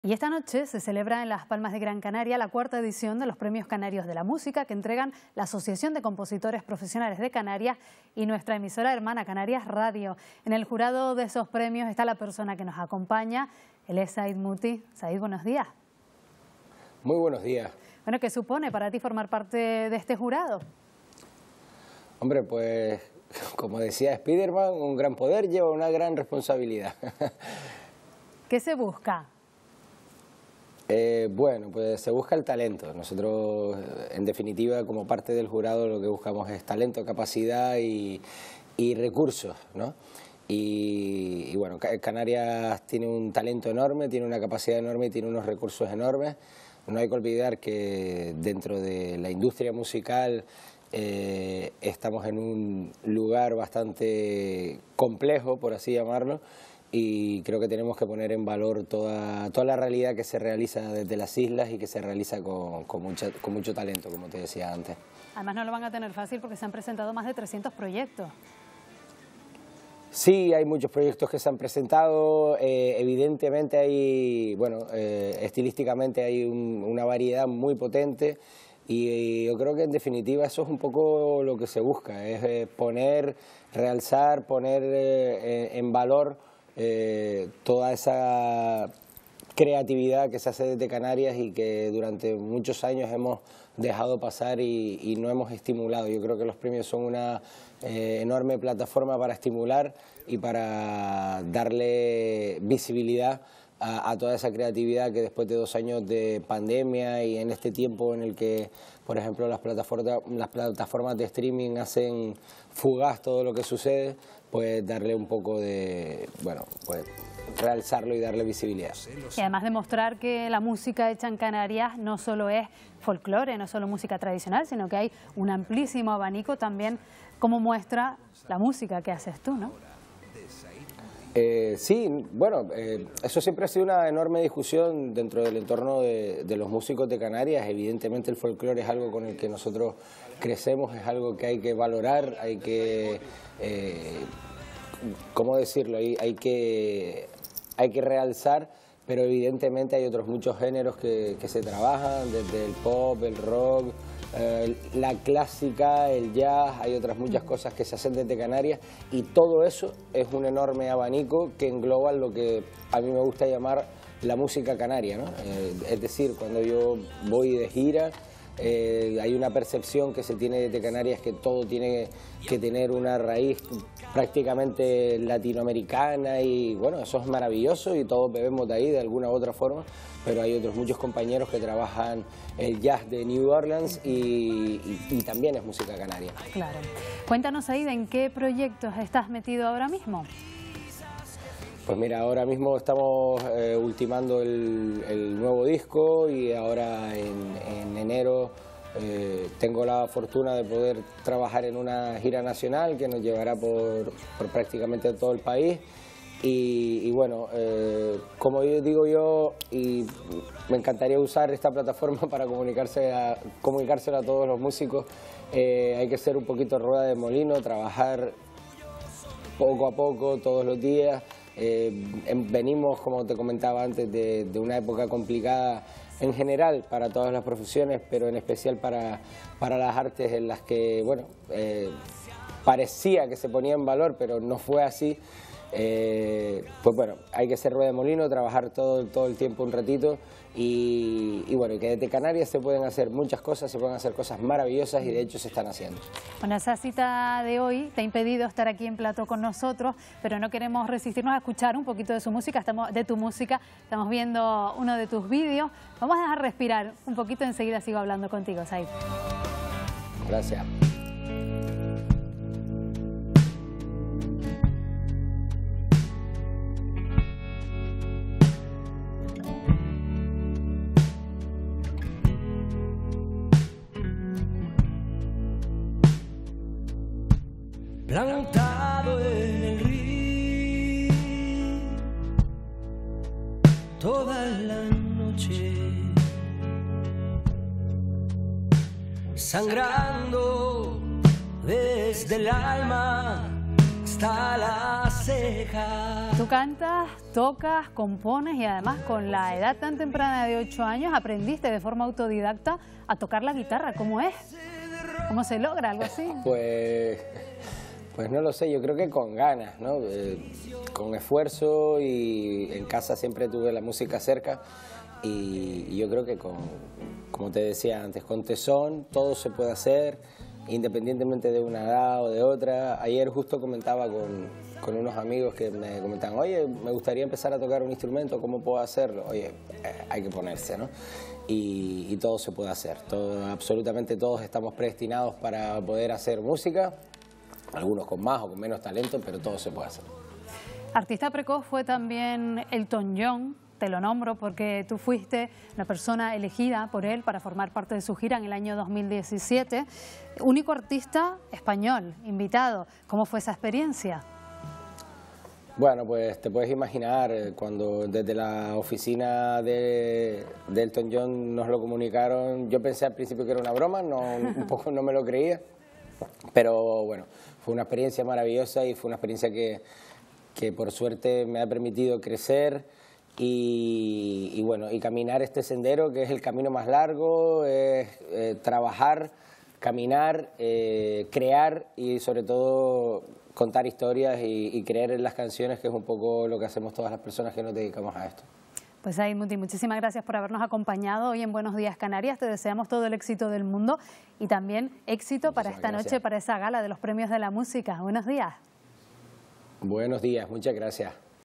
Said Muti en los Premios Canarios de la Música
La Asociación de compositores profesionales de Canarias y nuestra emisora hermana Canarias Radio hacen entrega de los Premios Canarios de la Música. En el jurado de esos premios está Said Muti.